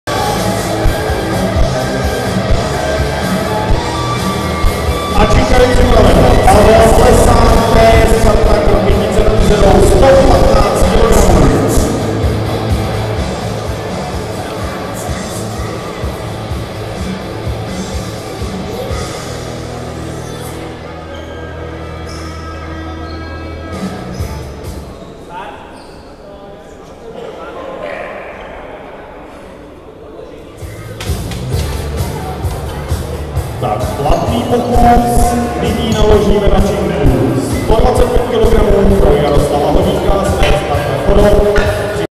Plesar Pavel (1960), BP2, 115 kg
12. Mistrovství ČR v benčpresu dorostu, juniorů a masters

Místo: Sportovní hala v areálu plaveckého bazénu, Jihlava

Federace: ČSST
Pořadatel: Powerlifting Jihlava